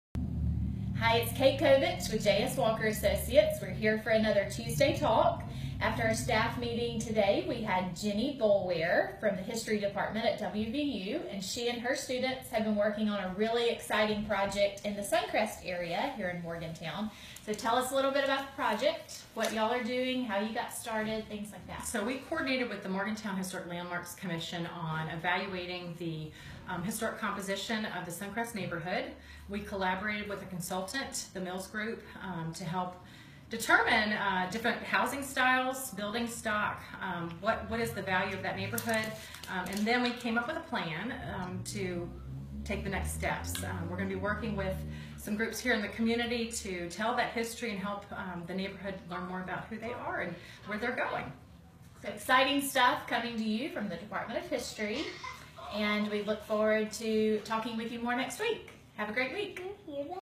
Tuesday Talks: Suncrest Project
Today, we were joined by Jenny Boulware from the WVU Department of History. She and her students have been researching the history and preservation of the Suncrest neighborhood. Suncrest is an area where we see great marketability and high home values due to the flat, large lots and proximity to Morgantown's amenities.